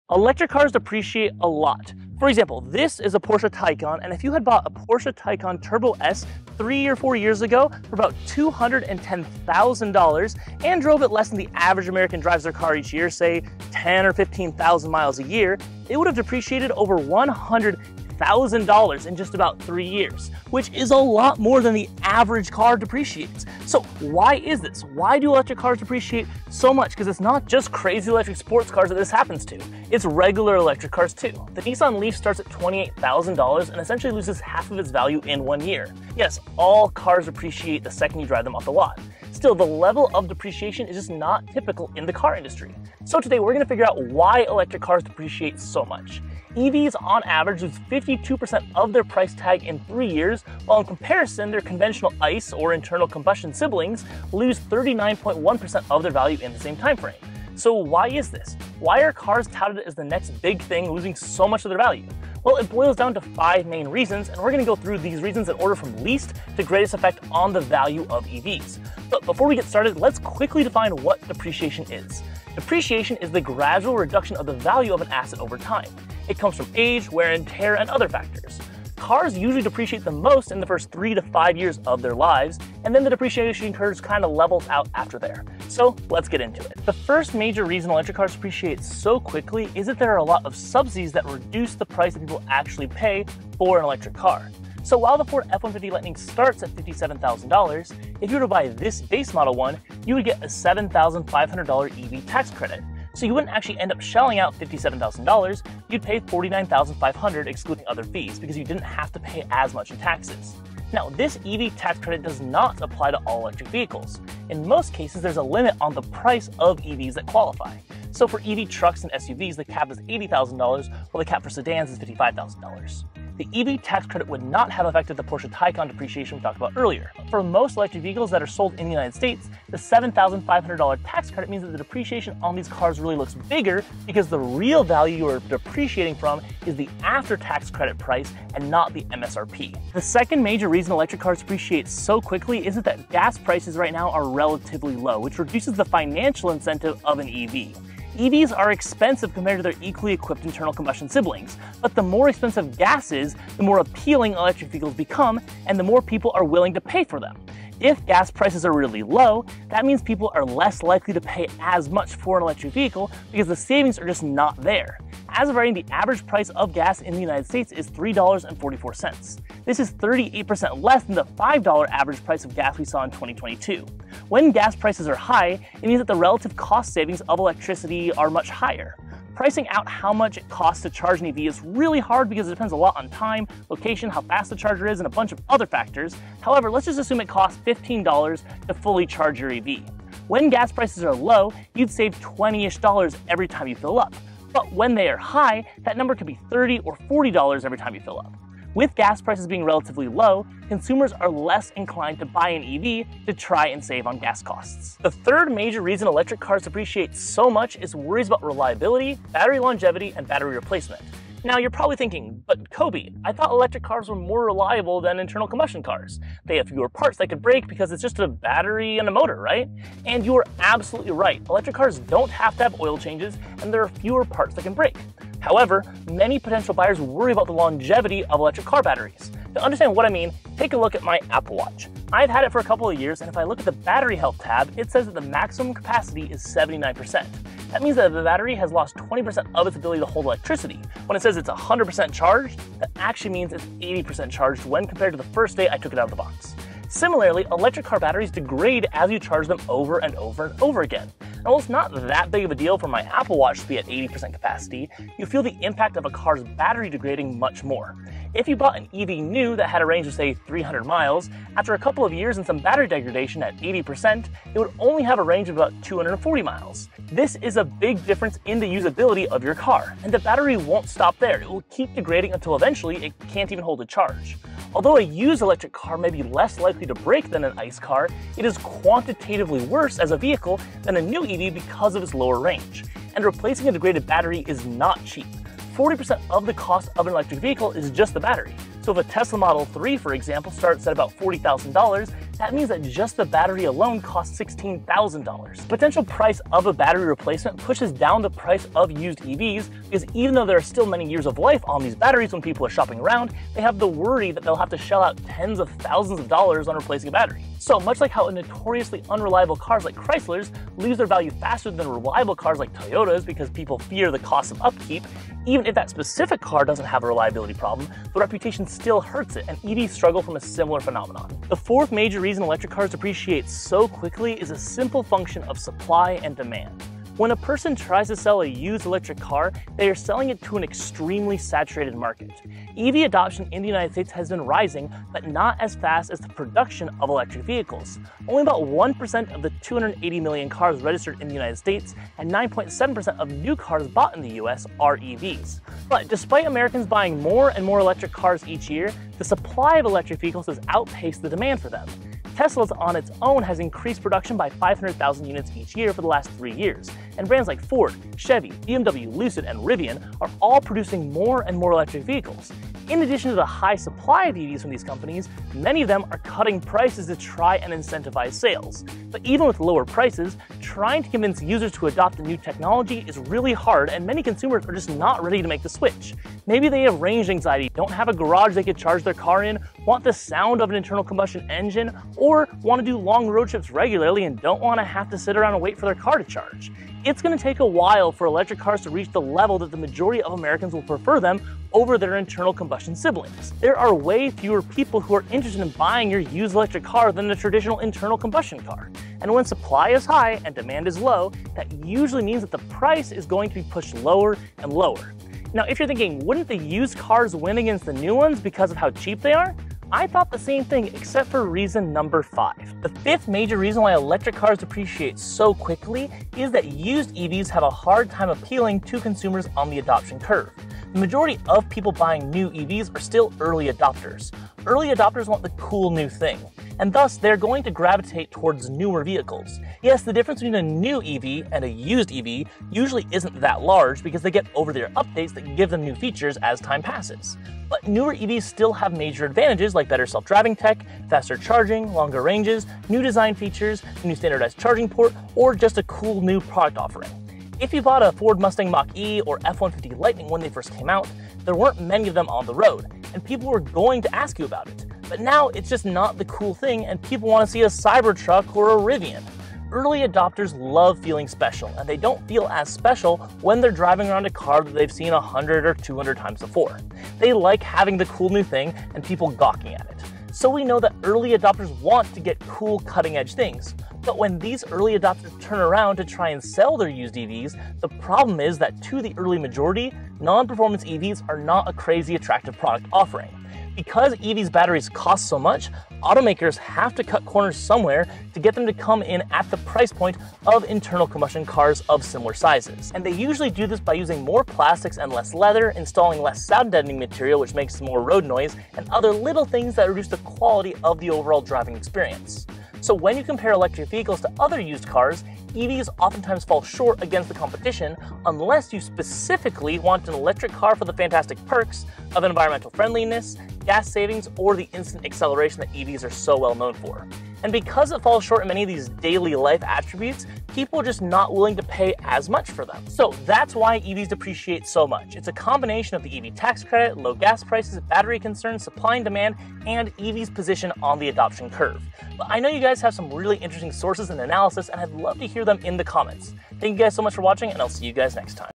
Why Do Electric Cars Depreciate So Fast?
Electric cars depreciate at an alarming rate, and it's not just high-end models like the Porsche Taycan. If you bought a Taycan Turbo S for $210,000 a few years ago and drove it less than the average 10,000 miles per year, it would have depreciated by over $100,000 - that's 50% in just three years! Even more affordable EVs like the Nissan Leaf, starting at $28,000, lose about half their value in one year.

In this video, we explore why EVs lose value so quickly. On average, electric vehicles depreciate 52% in three years, compared to 39.1% for traditional internal combustion engine (ICE) cars. We break down the reasons behind this trend, from EV tax credits and low gas prices to concerns about battery longevity, supply and demand dynamics, and the adoption curve of new technologies.

We start by defining depreciation and then delve into the five main factors driving EV depreciation, from subsidies and tax credits to market saturation and consumer preferences. Whether you're considering buying an EV or just curious about the economics behind them, this video will provide valuable insights.

Watch to understand the hidden costs of owning an electric vehicle and why their value drops so dramatically.

0:00 Intro
1:26 Reason One
2:22 Reason Two
3:30 Reason Three
5:20 Reason Four
6:52 Reason Five